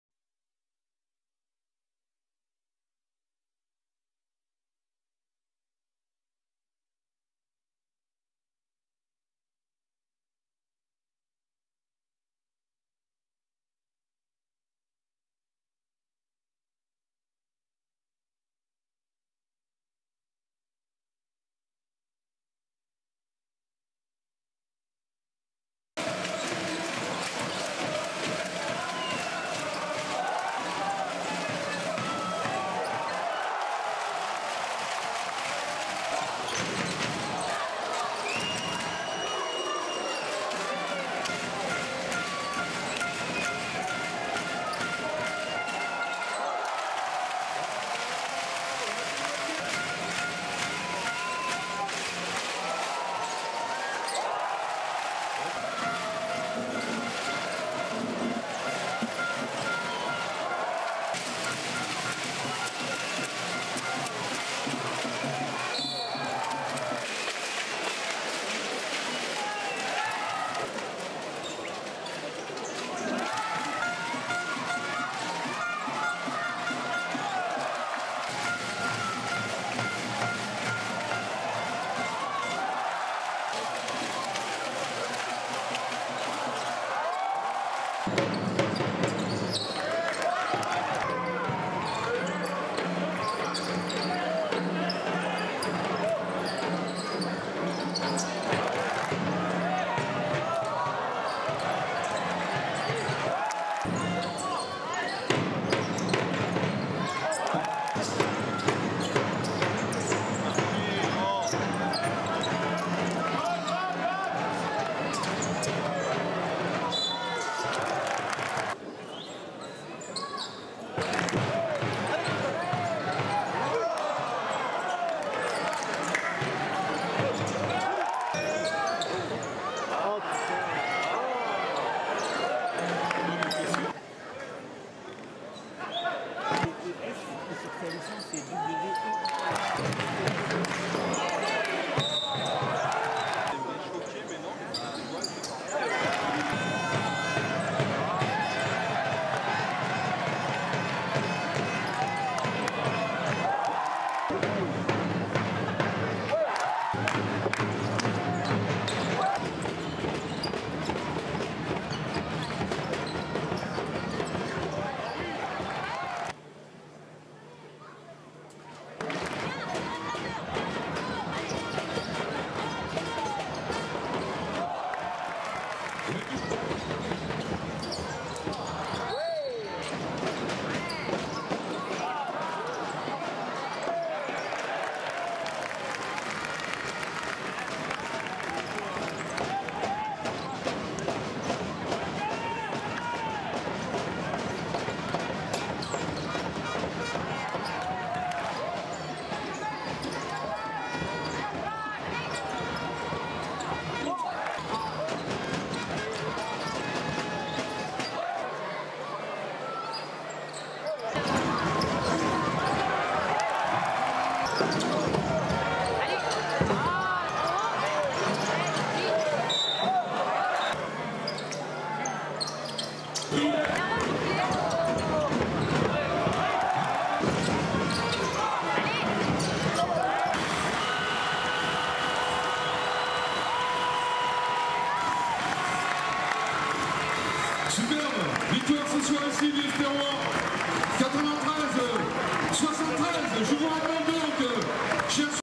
Wendell Mckines France Highlights
Wendell's first season as a professional proved to be a successful one as he averaged 17 points and 9 rebounds. Wendell also shot over %80 from the Free throw line as well as %60 from the field.